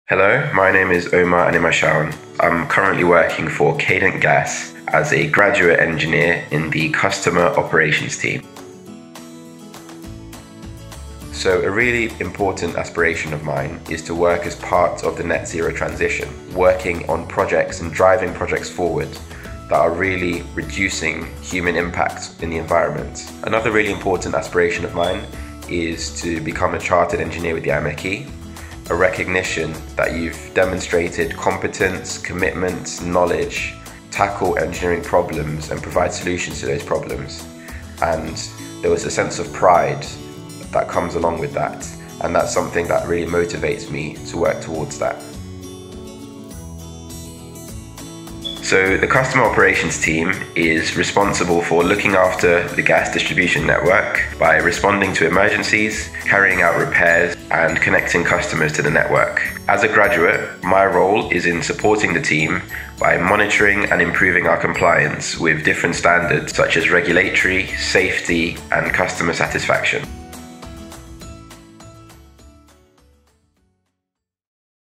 Meet Our Graduates: Omar Animashaun AMIMechE – My Career Aspirations
Omar Animashaun AMIMechE is a graduate engineer with Cadent Gas. Omar talks about his job role and his future career aspirations which include becoming a Chartered Engineer.